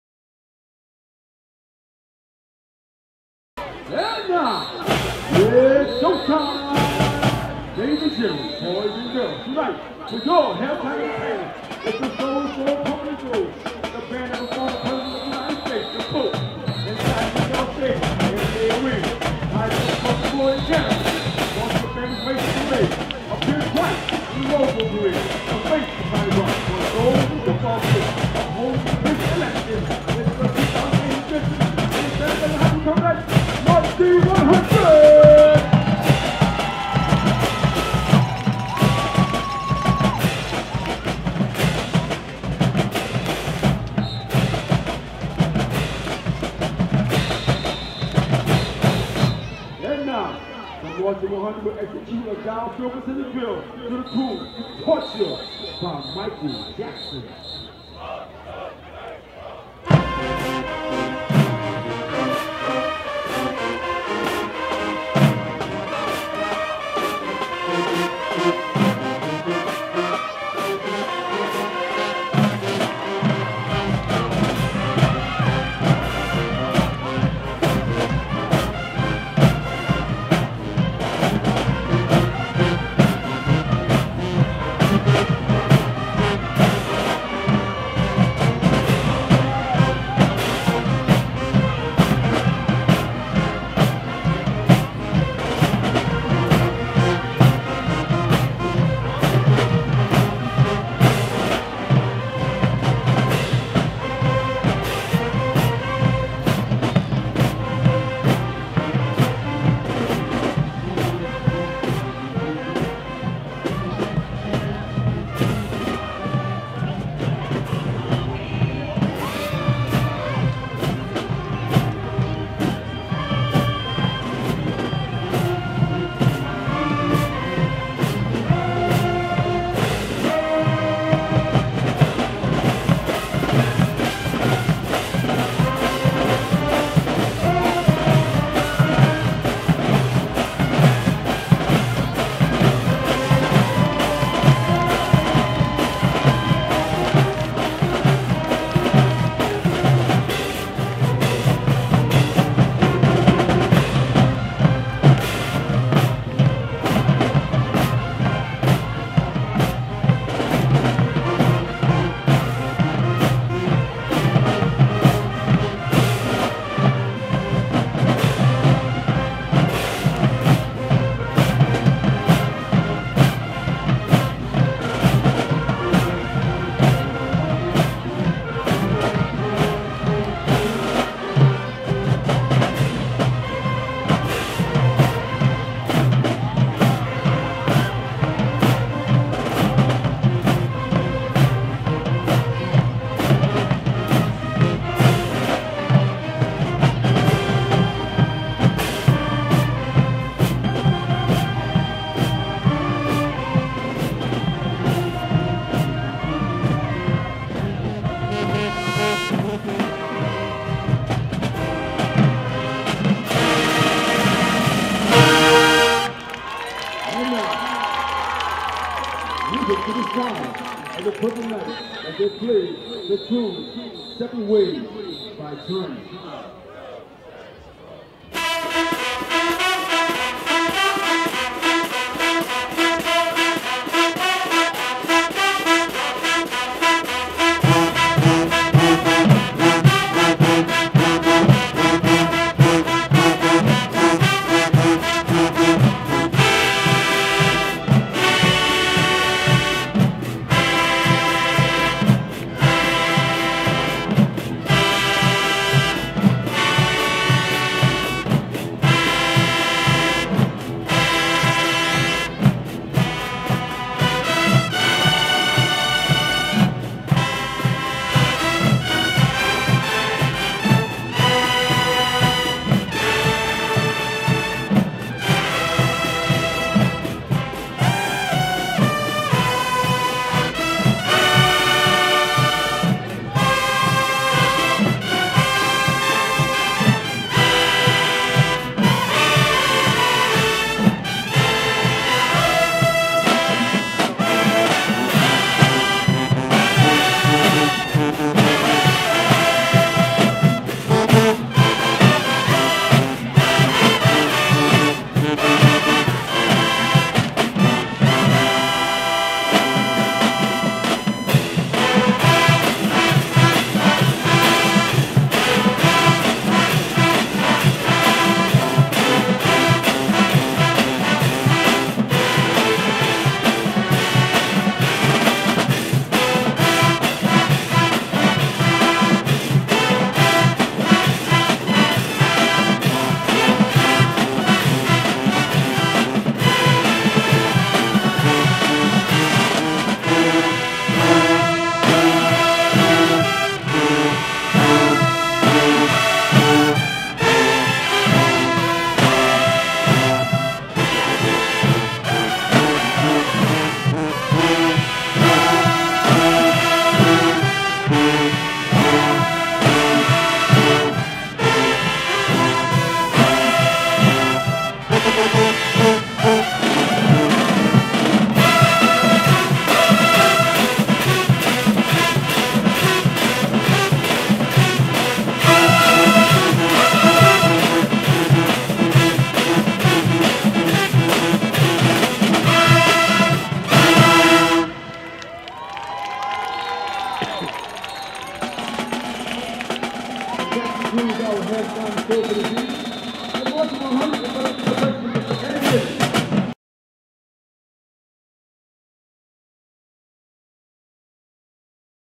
St Aug band halftime show 2018 @ Thibideuax game
St Aug Marching 100 Performing @ Halftime. Providing Coverage where needed. Great Season Still ahead.